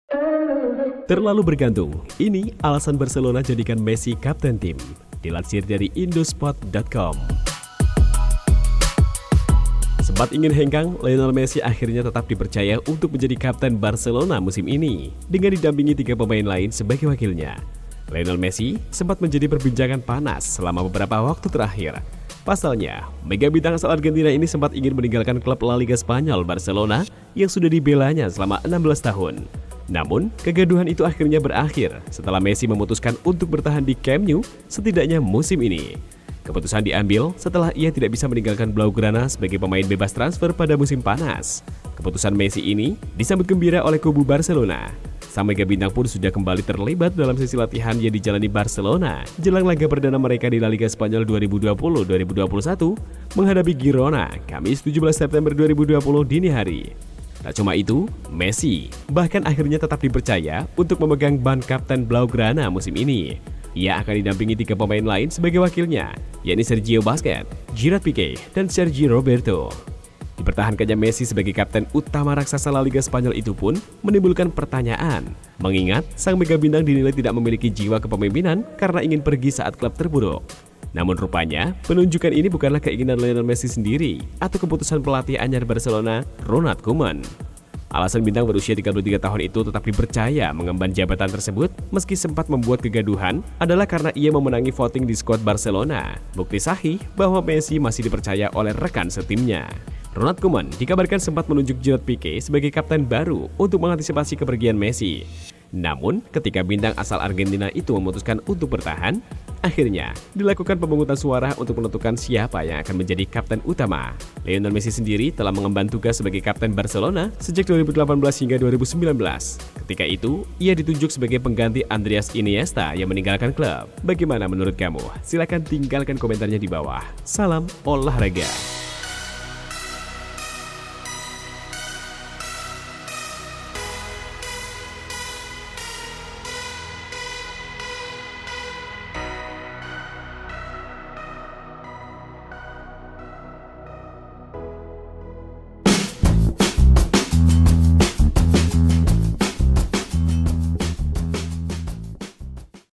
Terlalu Bergantung, Ini Alasan Barcelona Jadikan Messi Kapten Tim
Sempat ingin hengkang, Lionel Messi akhirnya tetap dipercaya untuk menjadi kapten Barcelona musim ini, dengan didampingi tiga pemain lain sebagai wakilnya.